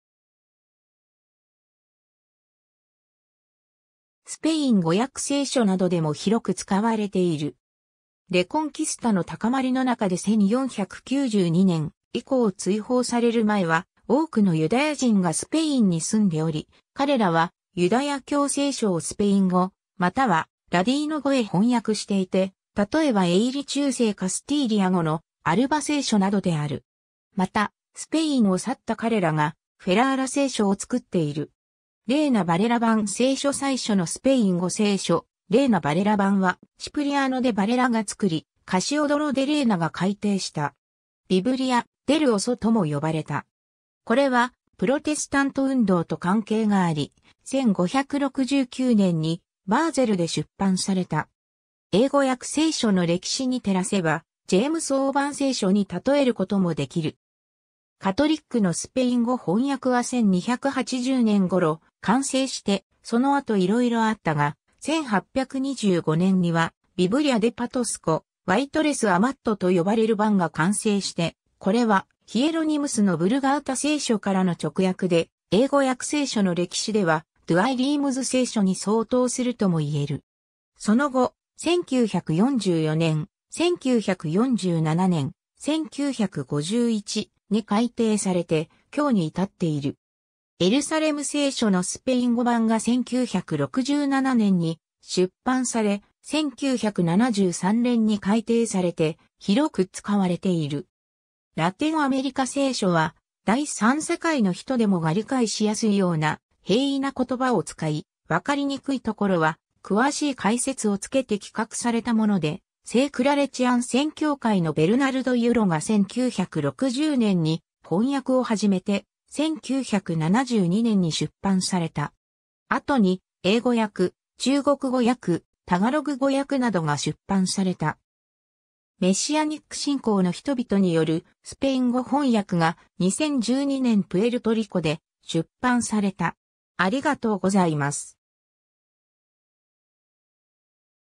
スペイン語訳聖書
スペイン語訳聖書（スペインごやくせいしょ、英語: Bible translations into Spanish)はキリスト教聖書のスペイン語への翻訳を扱う。スペイン語はスペインだけでなく、中南米諸国（ラテンアメリカ）などでも広く使われている。
 レコンキスタの高まりの中で1492年以降追放される前は多くのユダヤ人がスペインに住んでおり、彼らはユダヤ教聖書（旧約聖書）をスペイン語またはラディーノ語（スペイン在住ユダヤ人が使ったスペイン語方言）へ翻訳していて、例えば絵入り中世カスティーリア語の『アルバ聖書』（Biblia de Alba、1430年）などである。
また、スペインを去った彼らが『フェラーラ聖書』（Biblia de Ferrara、1553年）を作っている。
 『レイナ・ヴァレラ版』聖書（1569年） 最初のスペイン語聖書『レイナ・ヴァレラ版』（Reina-Valera）はシプリアノ・デ・ヴァレラ（Cipriano de Valera）が作り、カシオドロ・デ・レイナ（Casiodoro de Reina）が改訂した。
『ビブリア・デル・オソ』（Biblia del Oso）とも呼ばれた。
これはプロテスタント運動と関係があり、1569年にバーゼルで出版された。
英語訳聖書の歴史に照らせば『ジェームス王版聖書』に例えることもできる。
 カトリックのスペイン語翻訳は1280年ごろ完成して、その後いろいろあったが、1825年には『Biblia de Petisco y Torres Amat』と呼ばれる版が完成して、これはヒエロニムスのヴルガータ聖書（ラテン語）からの直訳で、英語訳聖書の歴史では『ドゥアイ・リームズ聖書』に相当するともいえる。
その後、1944年、1947年、1951に改訂されて、今日に至っている。
 『エルサレム聖書』のスペイン語版（Biblia de Jerusalén）が1967年に出版され、1973年に改訂されて、広く使われている。
 ラテンアメリカ聖書（La Biblia latinamericana）は第三世界の人でもが理解しやすいような平易な言葉を使い、分かりにくい所は詳しい解説を付けて企画されたもので、聖クラレチアン宣教会のベルナルド・ユロ（Bernardo Hurault）が1960年に翻訳を始めて、1972年に出版された。
後に英語訳（『クリスチャンコミュニティバイブル』）、中国語訳（『牧霊聖経』）、タガログ語訳などが出版された。
 メシアニック信仰の人々（キリスト教を信ずるユダヤ人たち）によるスペイン語翻訳が、2012年プエルトリコで出版された。